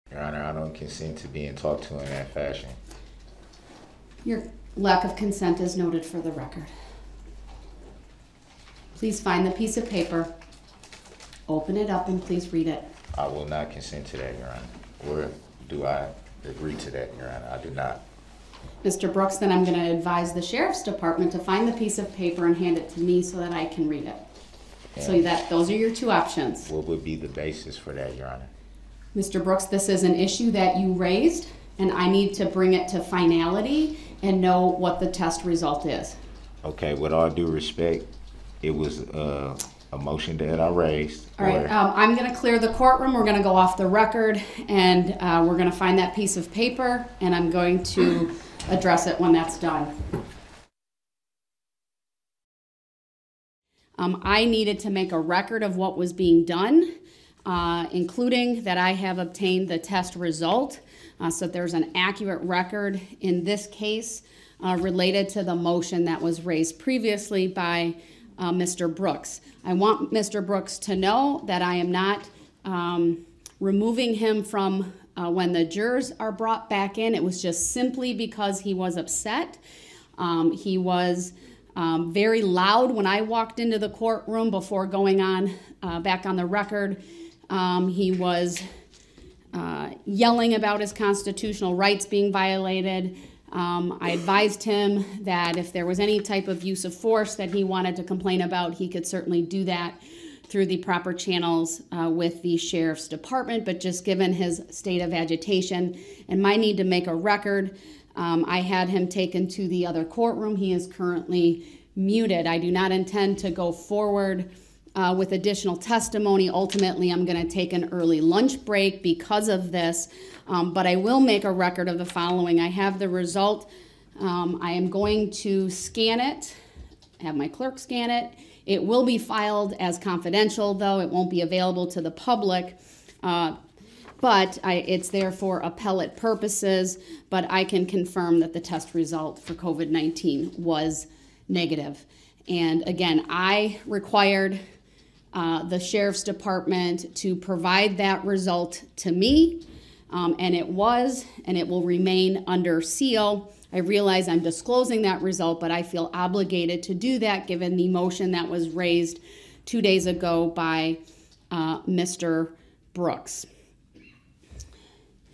Waukesha parade trial: Darrell Brooks refusal to read COVID test result led to altercation
Witness testimony continued Friday afternoon in the Darrell Brooks Jr. trial in the Waukesha Christmas Parade attack.

But as there has been all week, there was drama again in the courtroom. This time it centered around Brooks' COVID test results.

After he refused to read the result, an altercation ensued and Brooks was removed from the courtroom. Judge Jennifer Dorow ultimately read the negative COVID result.